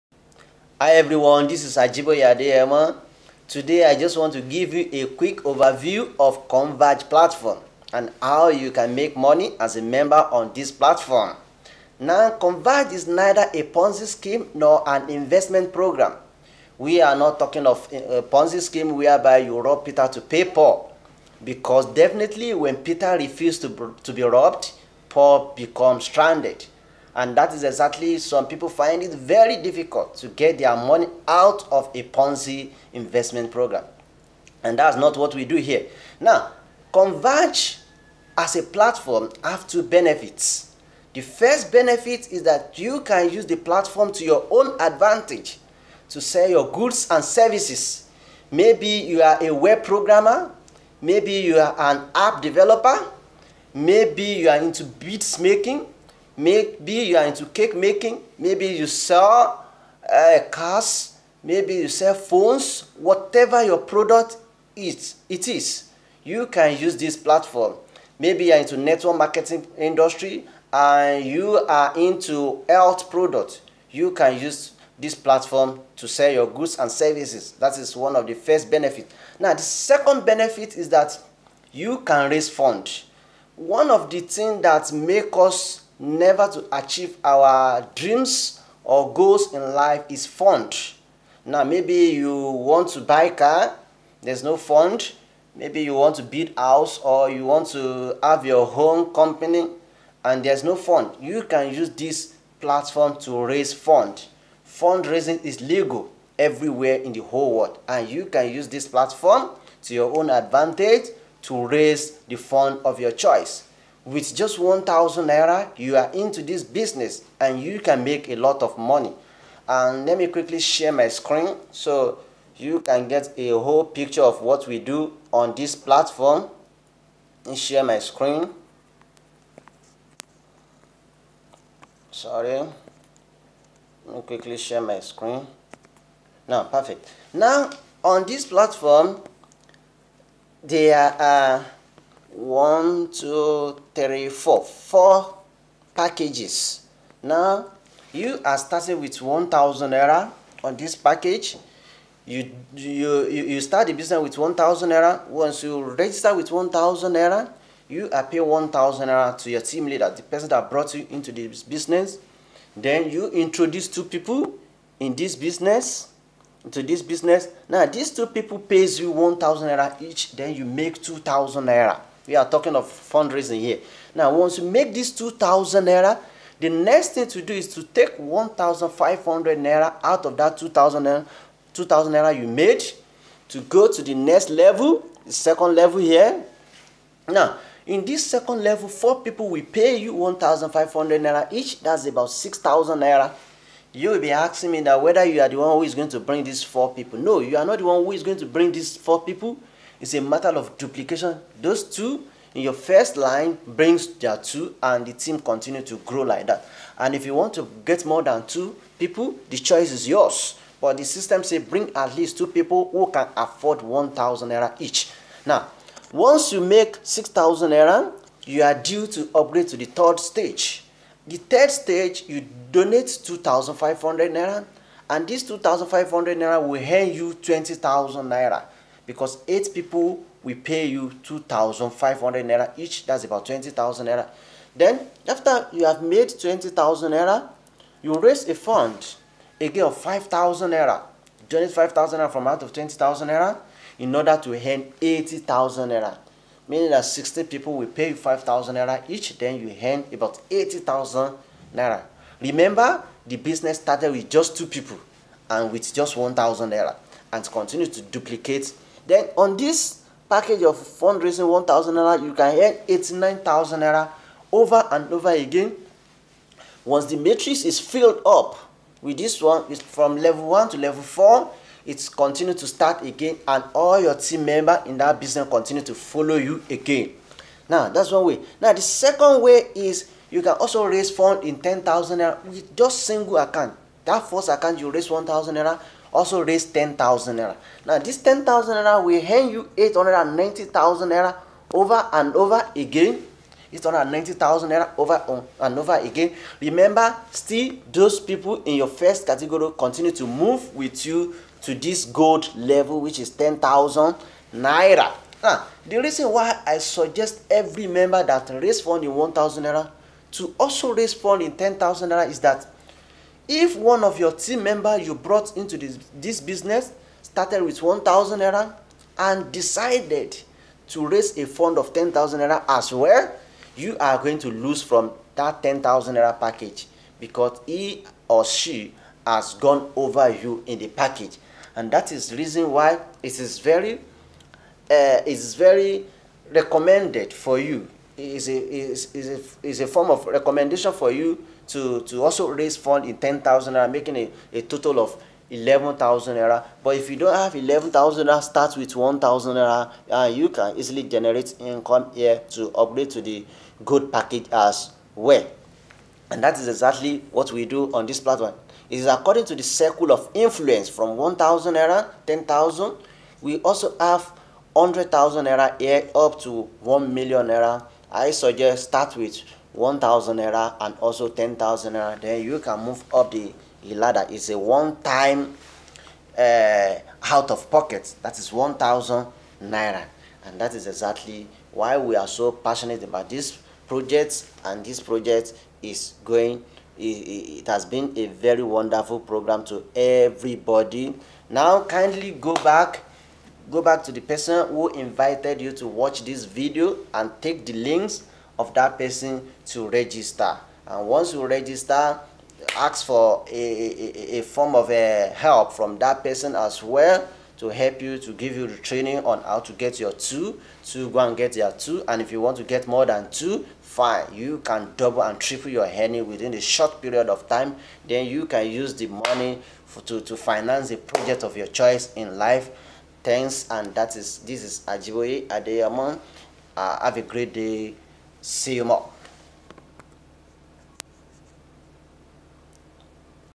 How Konvaj Works
This Opportunity is for Nigeria Only
How To Earn 89,000 Naira Over And Over Again
How To Raise Fund
Step 1.
Step 2. Login to your account ,Click on MENU top right corner and add your bank account , Then Click RAISE FUNDS
Step 3. Select Silver Zero (1000 naira)
Step 4. Follow the process and after done
Step 5. Click on Send payment, then click on ( details) to view the account number of who to pay, make payment immediately
Step 6. After making payment to the person, Click send payment again and click paid,

Note: You can do the same with Gold( 10,000) with the same account, simply click raise fund again after the first is confirmed